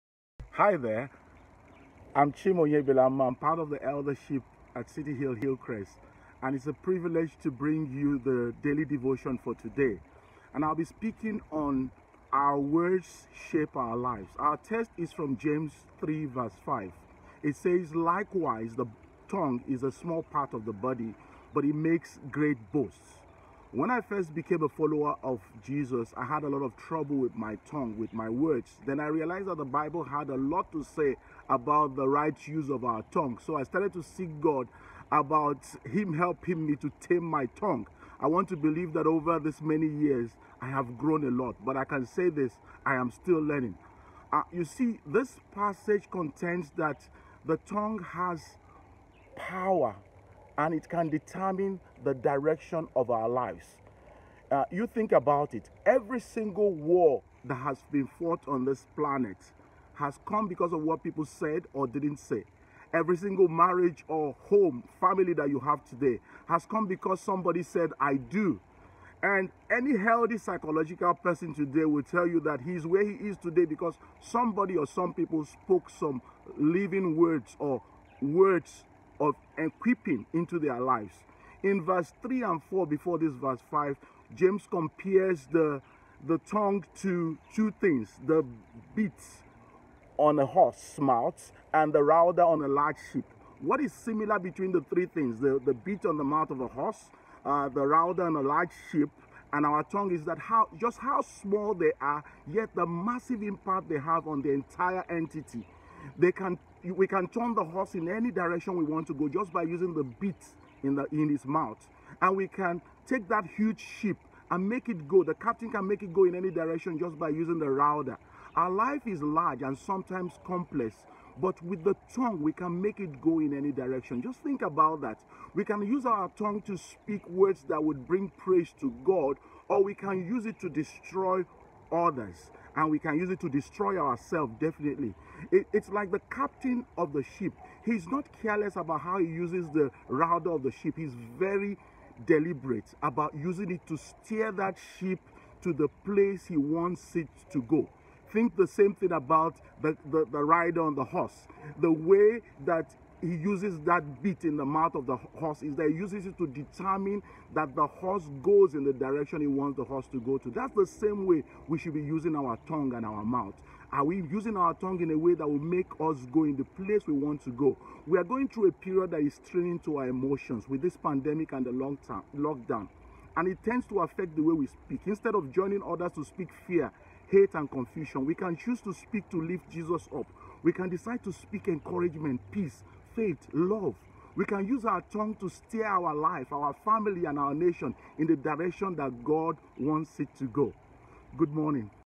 Our Words Shape Our Lives | Daily Devotion
Daily devotion with Chim.
In James 3:5, James says that the tongue has power and can determine the course of our lives. In verse 3 and 4, James compares the tongue to a bit in a horse’s mouth and the rudder of a large ship. Both these things are small in comparison to what they steer and yet they are necessary to steer the horse or ship. As the rider of a horse and the captain of a ship are intentional in the direction they choose, so we need to be intentional in the way we use our tongues and they things that we say.